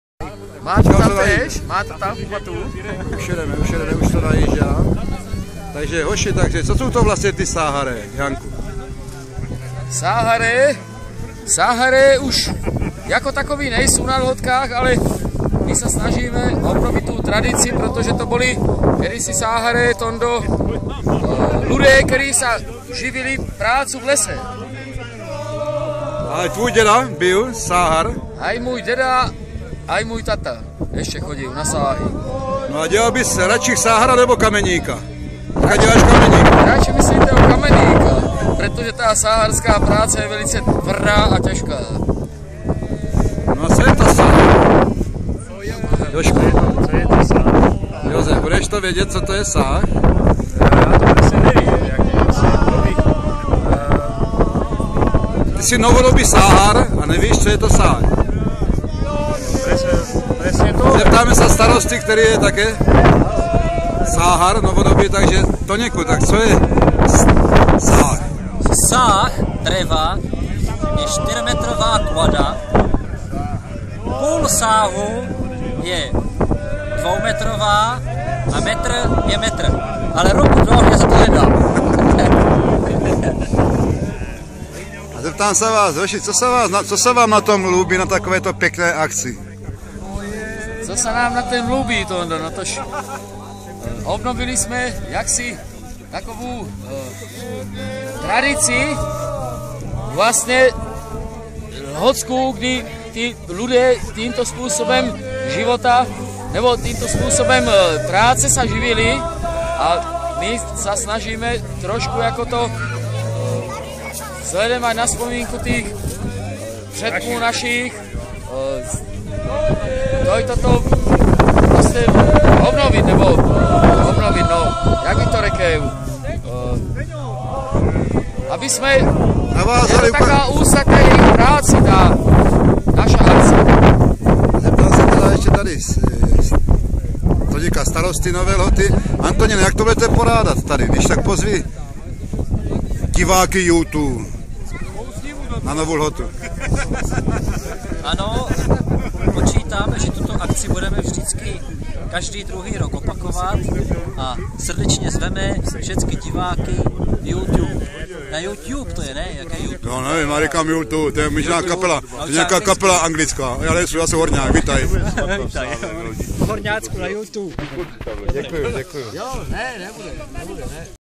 Ohlédnutí za "sáharama" na Nové Lhotě I.
K rekonstrukci starého řemesla v nejvýše položené obci Horňácka-Nové Lhotě přišlo 6. listopadu 2010. Byla to velmi vydařená  a zorganizovaná akce v přírodě Bílých Karpat. Příště se "sáharé"  opět sejdou  na podzim roku 2012.
(zaznamenáno na mobil Samsung S 8500 Wawe)